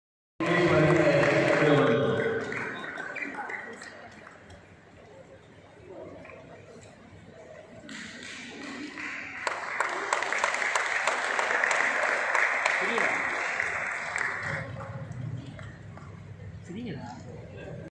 Sanjiv/nafeez ,runner up at state ranking,u13 doubles.Pray that they keep rising.🙏
Video from Rajarajan K